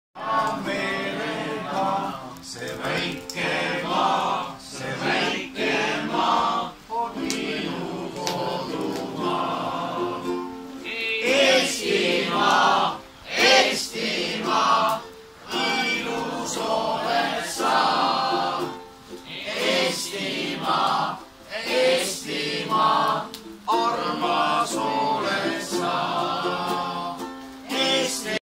Kontsert püstkojas
Improvisatsioonist tekkinud kontsert vihmasaju ajal Puhta vee teemapargis